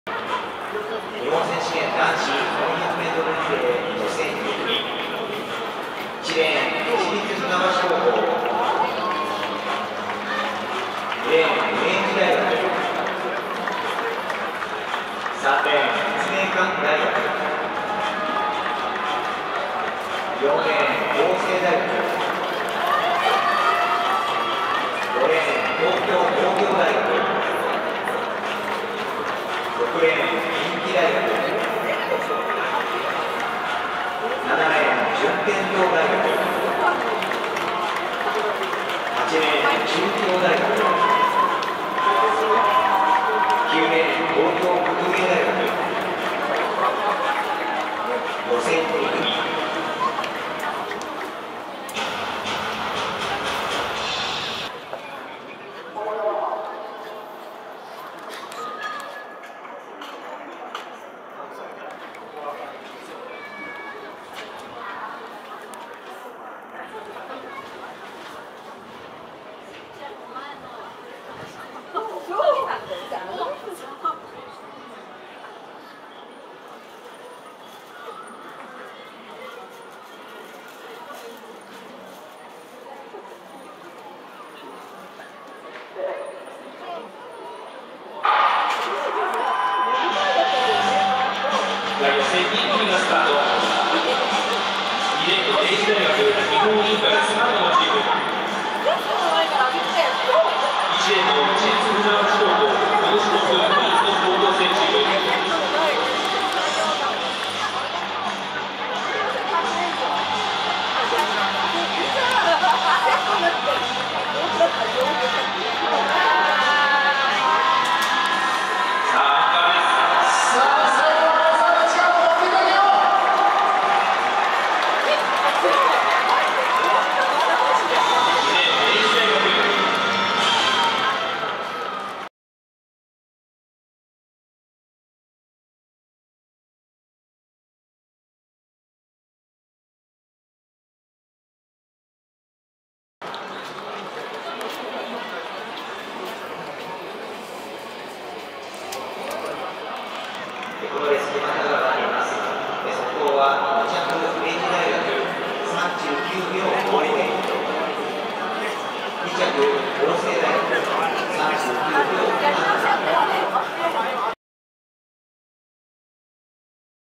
2023日本選手権リレー 男子4×100mリレー予選２組
2023日本選手権リレー、男子4×100mリレー予選２組。
1レーン　市立船橋高
2レーン　明治大
3レーン　立命館大
4レーン　法政大
5レーン　東京農業大
6レーン　近畿大
7レーン　順天堂大
8レーン　中京大
9レーン　東京学芸大